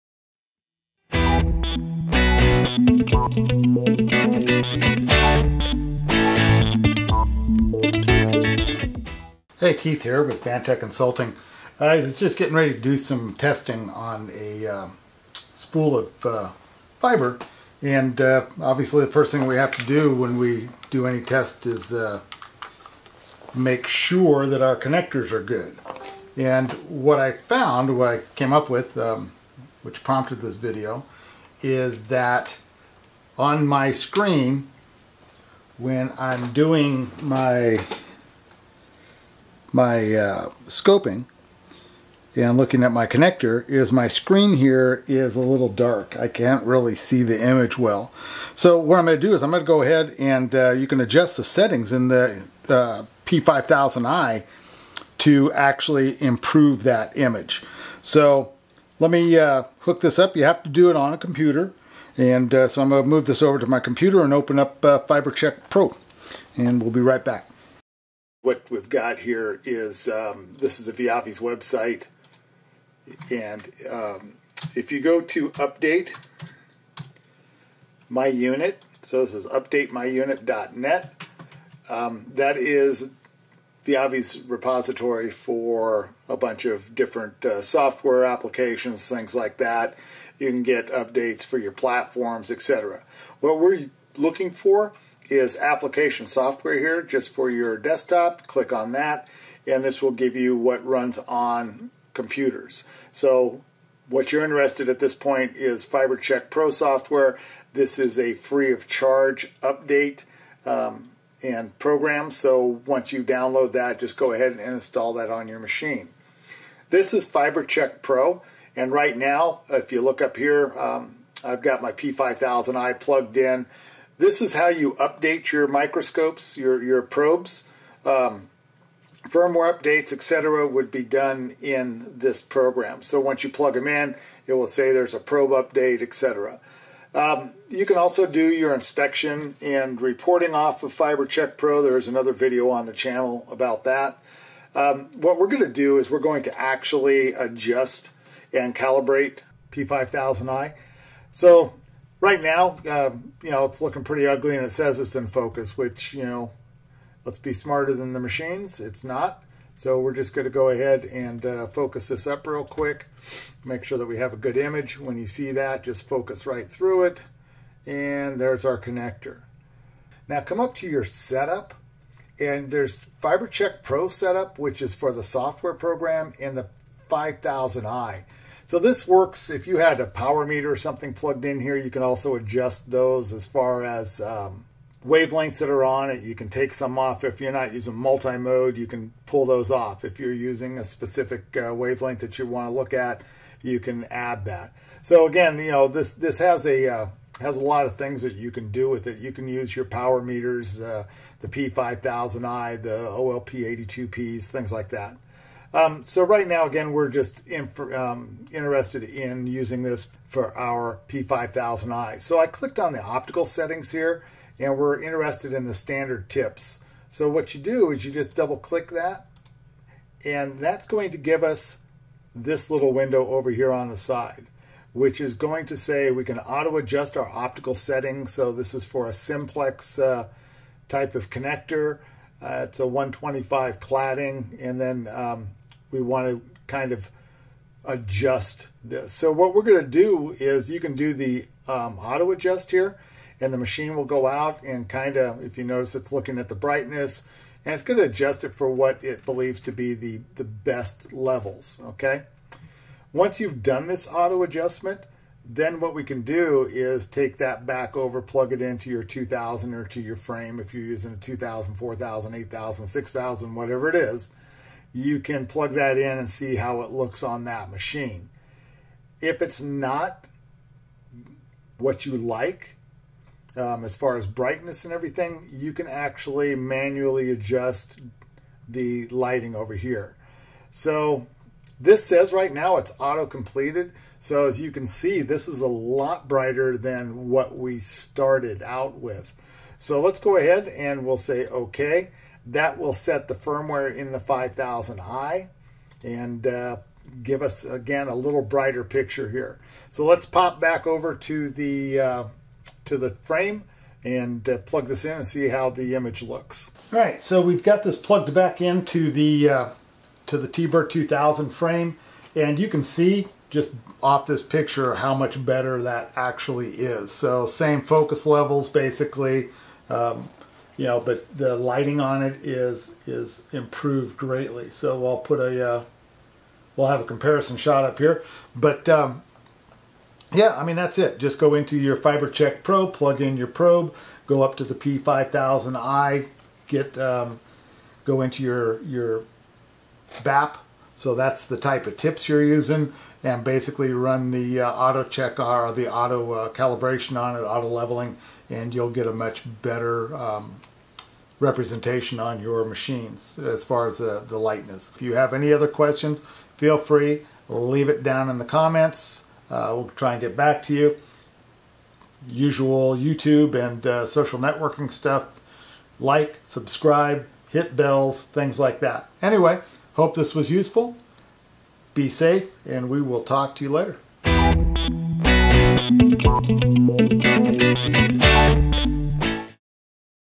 P5000i Image Calibration
Is your connector inspection screen too dark? Is it hard to see the end face of the connector? This video will show you how to calibrate the P5000i inspection probe using Viavi FiberChekPro software.

for online fiber and copper testing courses.